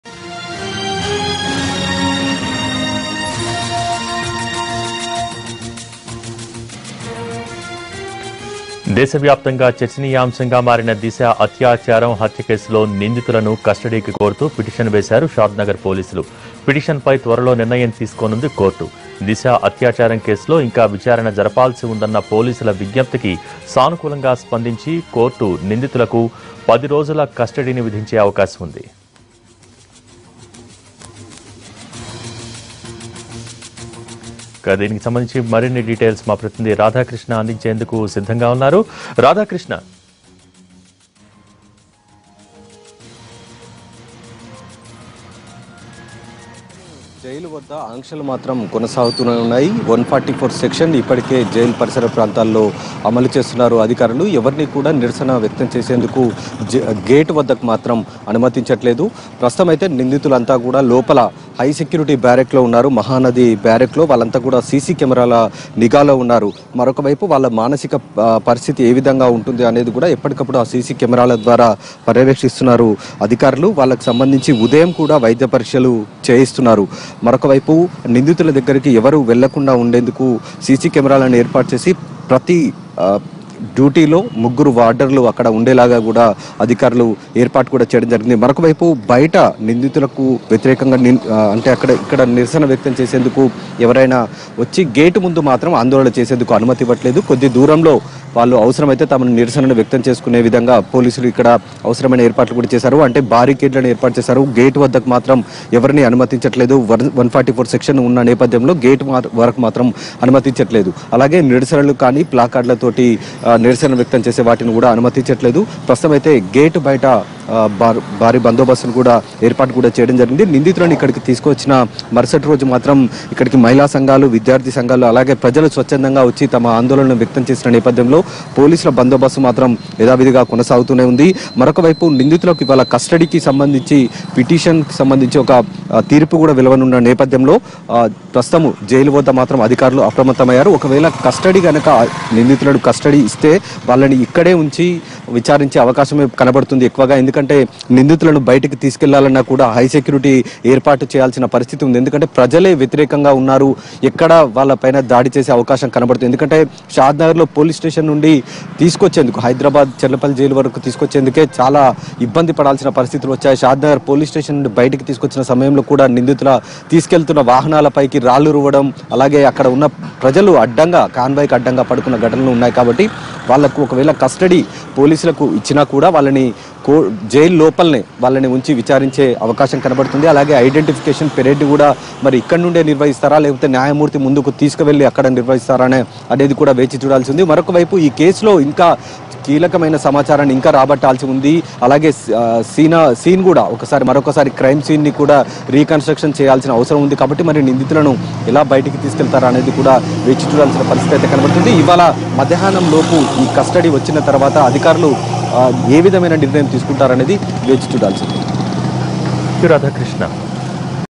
Shadnagar Court To Hear Petition On Disha Accused Custody || NTV
Shadnagar Court To Hear Petition Filed By Police Over Disha Accused Custody